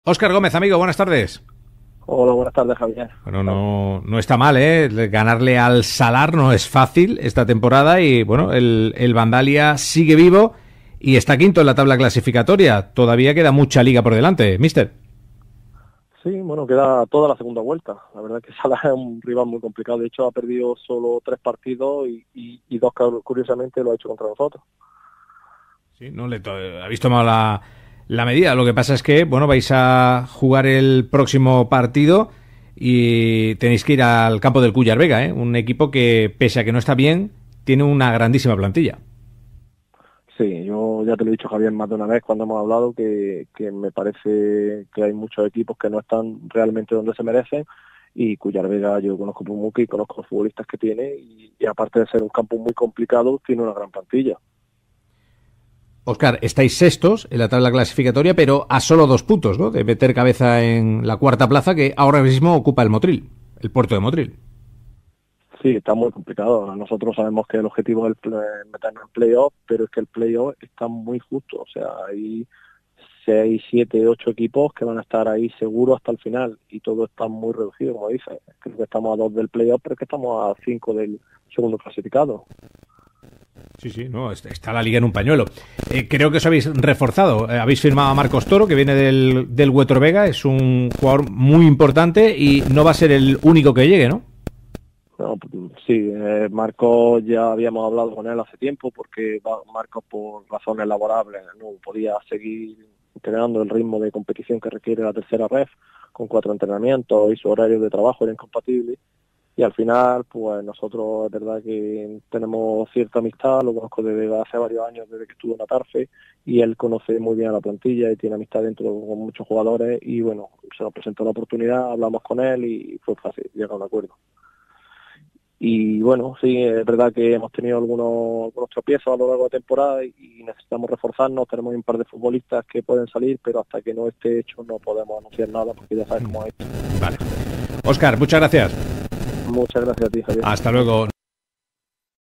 26-01-2023 Óscar Gómez
Entrevista al entrenador del Vandalia de Peligros en Primera Andaluza Sénior.